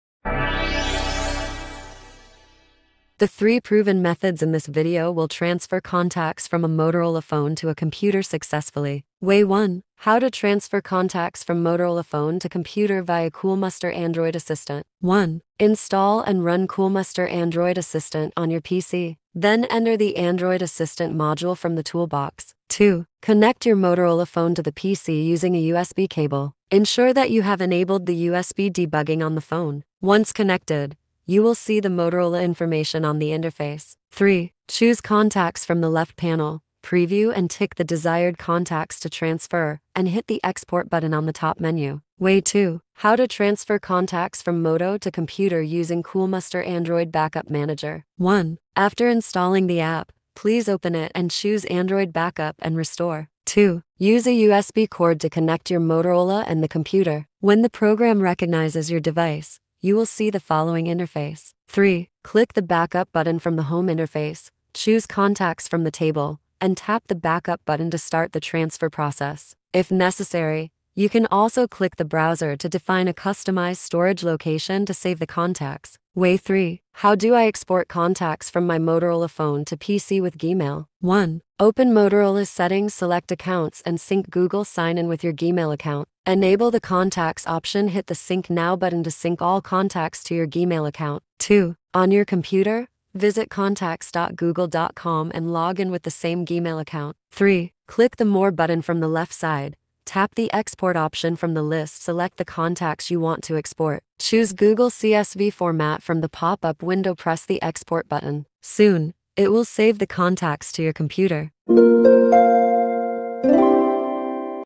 How to Transfer Contacts from Motorola Phone to Computer? [3 Ways]
How to transfer contacts from your Motorola phone to your computer effortlessly? If you are searching for reliable solutions, you can check the 3 quick ways in the video.

00:00 Start
00:09 Way 1. How to Transfer Contacts from Motorola Phone to Computer via Coolmuster Android Assistant
00:42 Way 2. How to Transfer Contacts from Moto to Computer Using Coolmuster Android Backup Manager
01:15 Way 3. How Do I Export Contacts from My Motorola Phone to my PC with Gmail